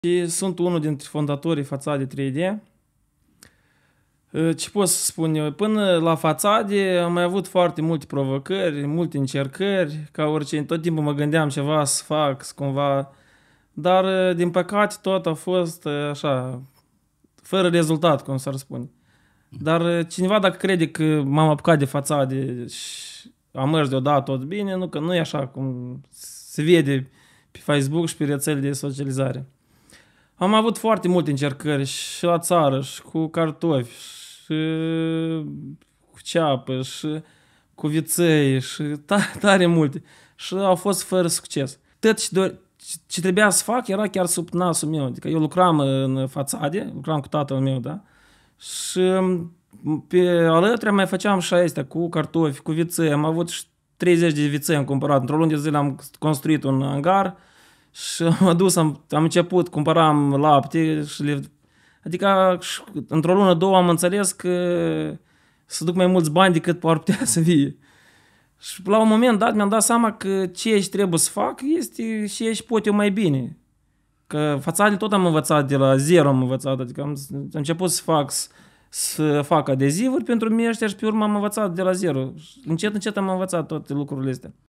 Încercările nereușite în afaceri
Urmărește acest video pentru a afla

- Cum dezvolți mai multe afaceri și de ce unele nu evoluează
- Afacerea trebuie făcută din pasiune

Dacă vrei să vizionezi întreg podcastul Provocarea de a fi antreprenor 👉 urmărește lansarea video-ului integral pe 09 iunie 2023.